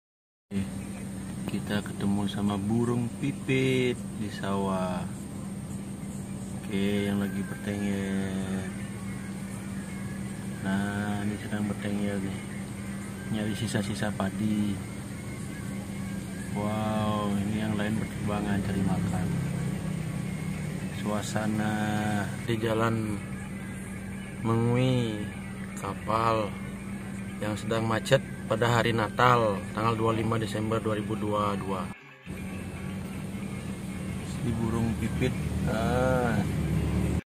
Tenang burung prit, sawah tetangga sudah mulai "nandur" nikmati saja padi santan muda yang tumbuh
Edited by VideoGuru:https, Copyright by @aajiptv